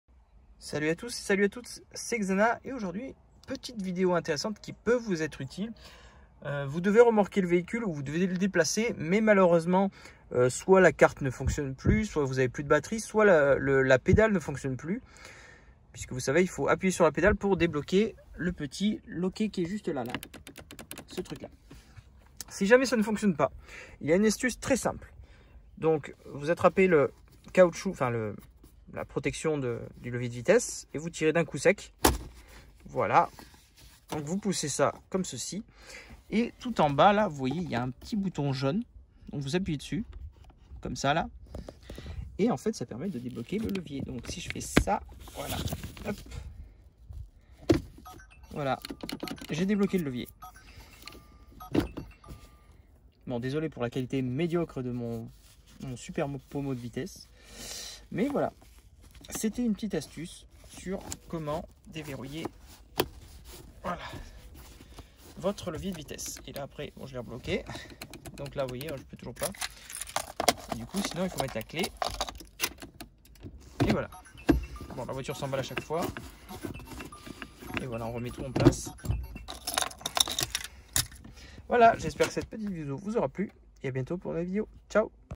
Renault Zoé débloqué le Levier de vitesse
Votre Zoé et bloqué en position Parking a cause d'une panne batterie ou d'un souci de capteur de pédale cette astuce vous permettra de débloqué le levier pour remorquage par exemple.